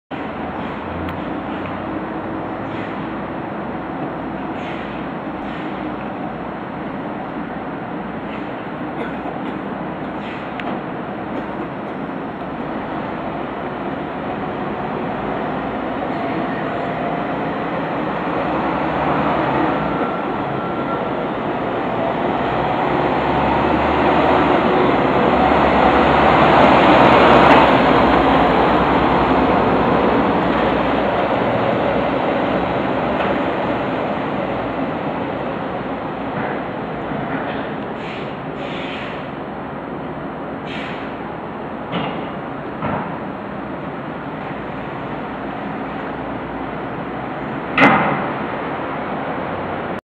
2013 LIEBHERR LTM 1220-5.2, RKM CRANE No. 264, B.C., departing at 8:45 PM on March 15, 2018
Time: 8:45pm. Ballast: 54t, in 5 parts. Location: HUDSON PLACE ONE 25-storey Highrise project by TOWNLINE, 777 Herald Street, Victoria, B.C., Canada. Job: Red WOLFF hammerhead tower crane, from M.R. CRANE SERVICES LTD., up. Mobile crane used its 12.2 meter fly jib on this job.